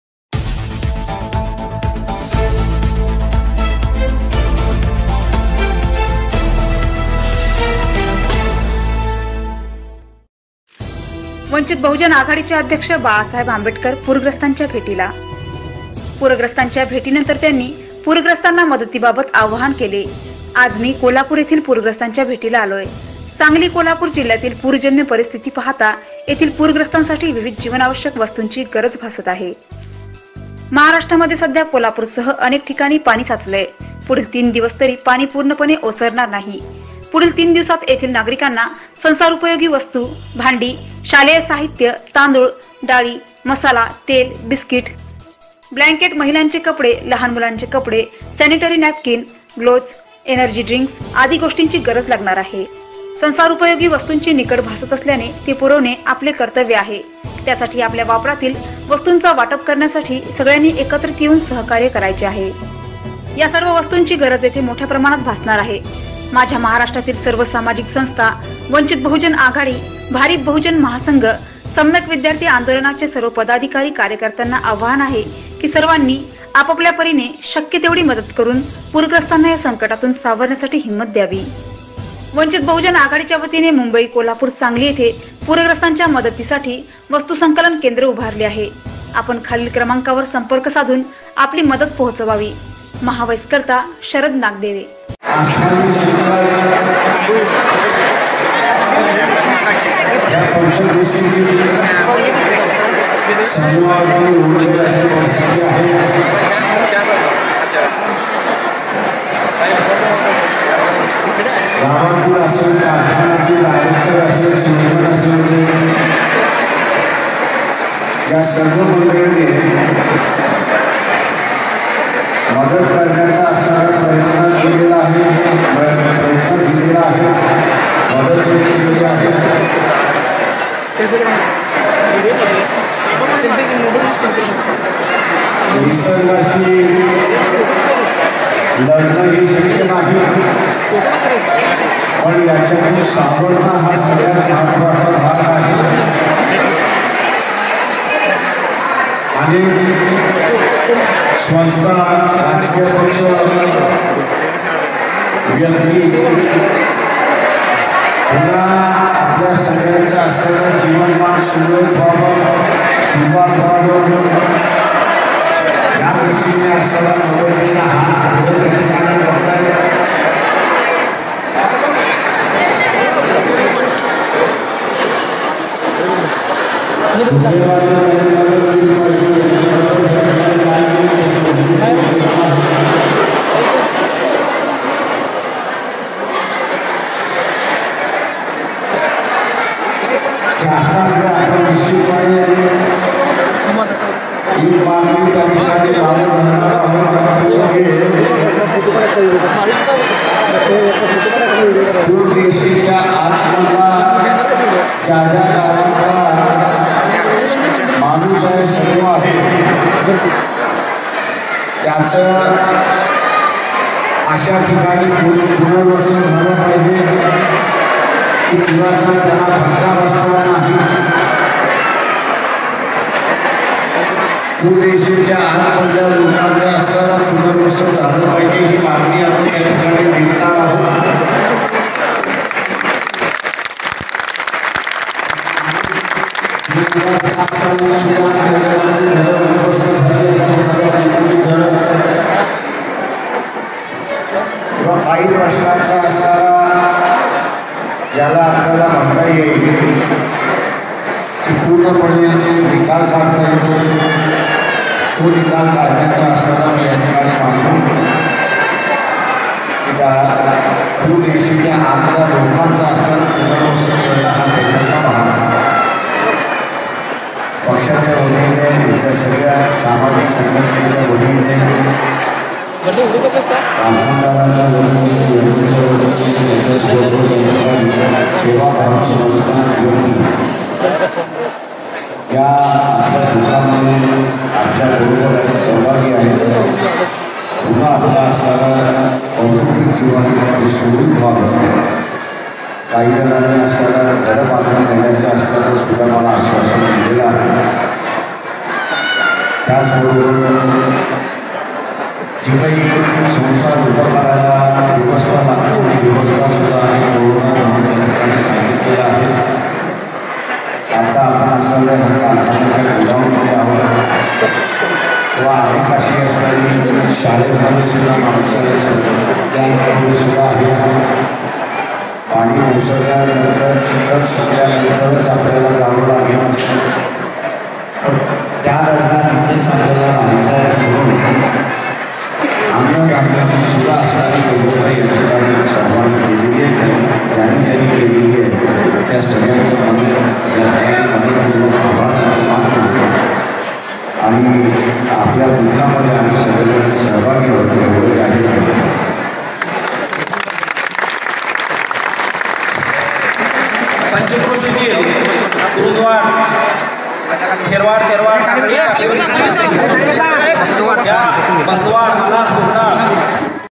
बाळासाहेब आंबेडकर पुरग्रस्तांचा भेटीला....
पुरग्रस्तांचा भेटी नतंर त्यांनी पुरग्रस्तांना मदती बाबत केलेले आव्हान आज मी, कोल्हापुर येथील पुरग्रस्तांच्या भेटीला आलोय. सांगली, कोल्हापूर जिल्ह्यातील पूरजन्य परिस्थिती पाहता तेथील पुरग्रस्तांसाठी विविध जीवनावश्यक वस्तूंची गरज भासत आहे.
महाराष्ट्र
 सध्या कोल्हापुरसह अनेक ठिकाणी पाणी साचलयं. पुढील तीन दिवस तरी पाणी पूर्णपणे ओसरणार नाही. पुढील तीन दिवसांत येथील नागरिकांना संसार उपयोगी वस्तू, भांडी, शालेय साहित्य, तांदूळ, डाळी, मसाला, तेल, बिस्कीट, ब्लँकेट, महिलांचे कपडे, लहान मुलांचे कपडे, सँनिटरी नँपकिन, ग्लोज, एनर्जी ड्रिंक आदी गोष्टींची गरज लागणार आहे.

संसार उपयोगी वस्तूंची निकड भासत असल्याने ती पुरविणे आपले कर्तव्य आहे. त्यासाठी आपल्या वापरातील इत्यादी वस्तूंची वाटप करण्यासाठी सगळ्यांनी एकत्र येऊन सहकार्य करायचे आहे. या सर्व वस्तूंची गरज तेथे मोठ्या प्रमाणात भासणार आहे.

माझी महाराष्ट्रातील सर्व सामाजिक संस्था, वंचित बहुजन आघाडी, भारिप बहुजन महासंघ, सम्यक विद्यार्थी आंदोलनाचे सर्व पदाधिकारी, कार्यकर्त्यांना आवाहन आहे की, सर्वांनी आपल्यापरीने शक्य तेवढी मदत करून पूरग्रस्तांना या संकटातून सावरण्यासाठी हिम्मत द्यावी.

वंचित बहुजन आघाडीच्यावतीने मुंबई, कोल्हापुर, सांगली येथे पुरग्रस्तांच्या मदतीसाठी वस्तू संकलन केंद्र उभारले आहे. आपण खालील क्रमांकावर संपर्क साधून आपली मदत पोहचवावी.
प्रकाश आंबेडकर

महाव्हॉईस करिता शरद नागदेवे.